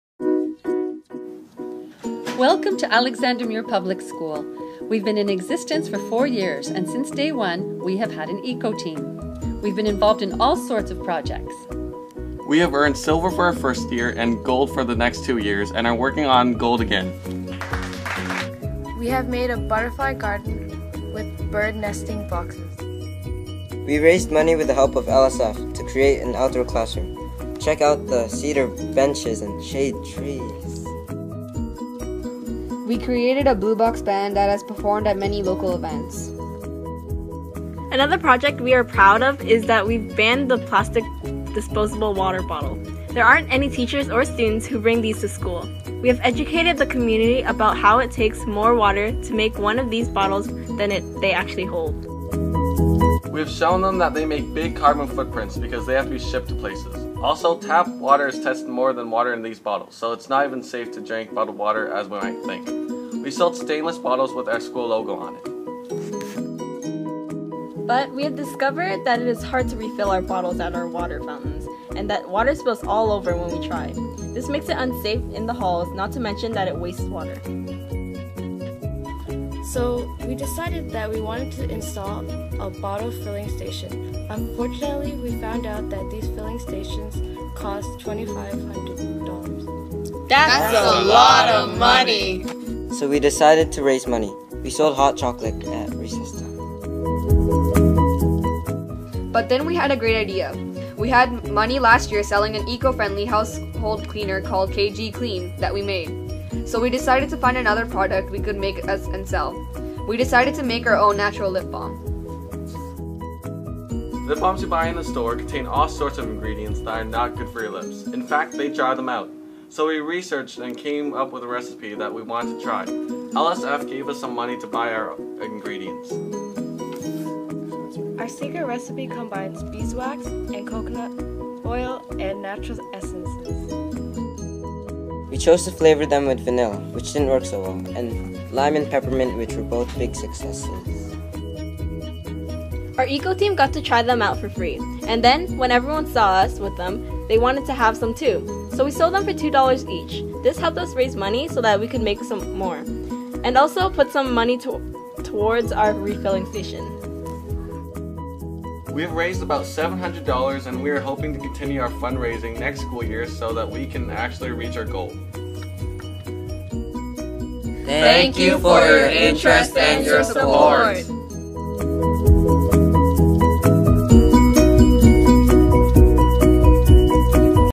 Alexander Muir Public School - Jack Layton Award Runner-Up 2012
Concerned about the harmful effects of conventional cleaning products and lip balm, the students in the Alexander Muir Public School Eco-Team made their own all-natural cleaning product (K-G Clean) and lip balm (Alexander Muir's Ecotastic Lip Balm).  They used the proceeds from the sales of these products to save for a water filling station which would accommodate the refilling of their reusable water bottles.  In previous years, this school has also created a butterfly garden with bird nesting boxes, an outdoor classroom, a blue box band, and banned disposable water bottles from their school.

Created by students from Alexander Muir Public School as part of their submission for the Learning for a Sustainable Future Jack Layton Award for Youth Action in Sustainability.